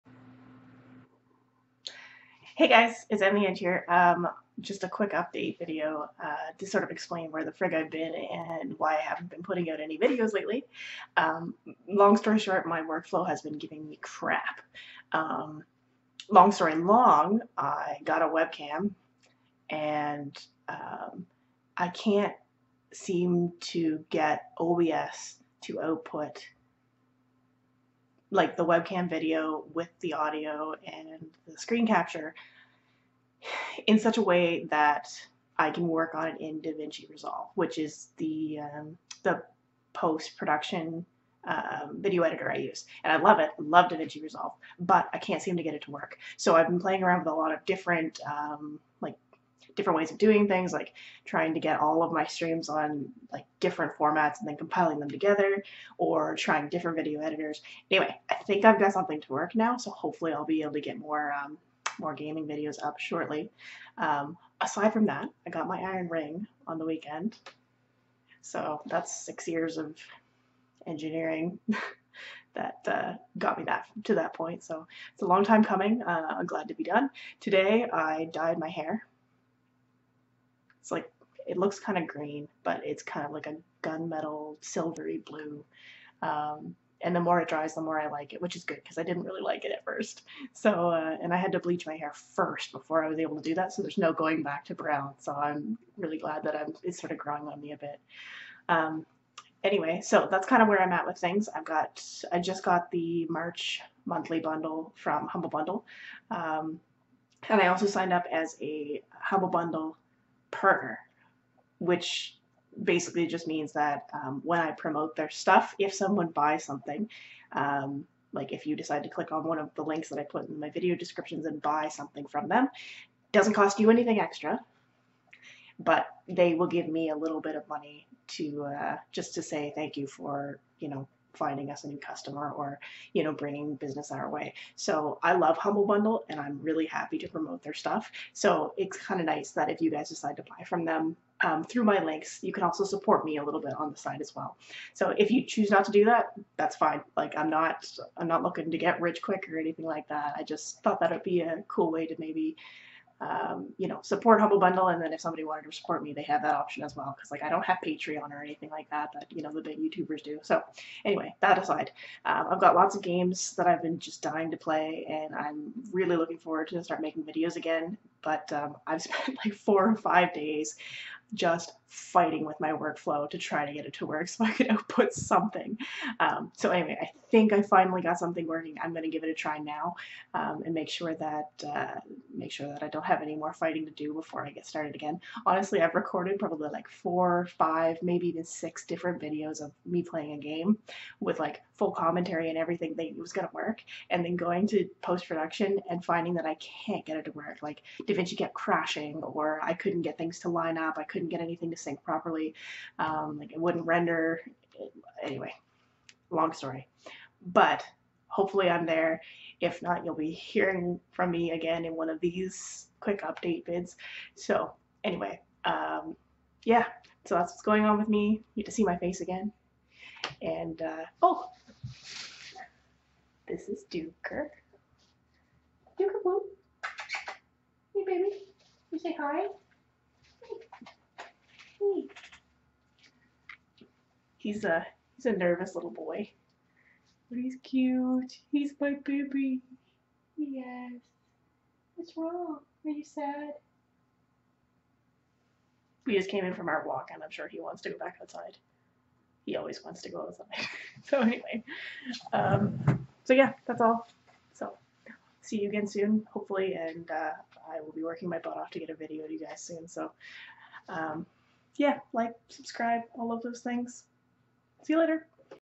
Quick Update | Iron Ring, Hair Dye, and Workflow Issues
Hey, guys! I've been having issues with my workflow lately so I thought I'd give a quick update on what's been going on.

Firstly: I got my Iron Ring on Saturday! (If you don't know what that is, check out this Wikipedia article

Secondly: I dyed my hair today!

Thirdly (and probably most importantly): I got a webcam and ever since then, my workflow for recording videos has been giving me loads of grief. I've been having compatibility issues up the ying-yang and it's made it impossible to get any gaming videos uploaded. Sorry about that! But fear not, dear reader, for I am working my butt off to get things up and running again and I hope to have some videos ready for upload tomorrow (fingers crossed)!